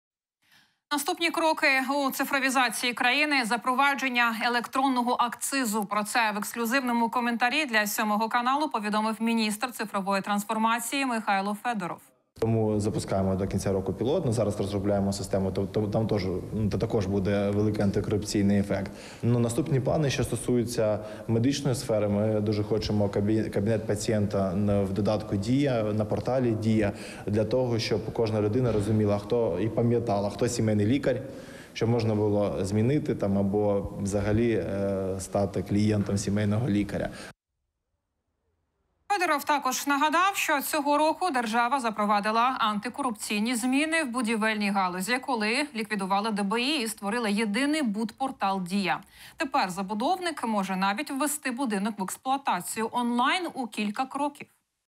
Мінцифри незабаром запровадить електронний акциз
Наступні кроки у цифровізації країни — запровадження електронного акцизу.

Про це в ексклюзивному коментарі для 7 каналу повідомив міністр цифрової трансформації України Михайло Федоров.

«Зараз запускаємо до кінця року систему е-акцизу, пілотуємо, там також буде великий антикорупційний ефект. Наступні плани, що стосується медсфери, ми дуже хочемо кабінет пацієнта в додатку на порталі Дія запровадити, щоб кожна людина пам’ятала, хто сімейний лікар, щоб можна було змінити чи взагалі стати клієнтом сімейного лікаря», — заявив Михайло Федоров.

Федоров також нагадав, що цього року держава запровадила антикорупційні зміни в будівельній галузі, коли ліквідувала ДАБІ й створила єдиний будпортал Дія. Тепер забудовник може навіть ввести будинок в експлуатацію онлайн у кілька кроків.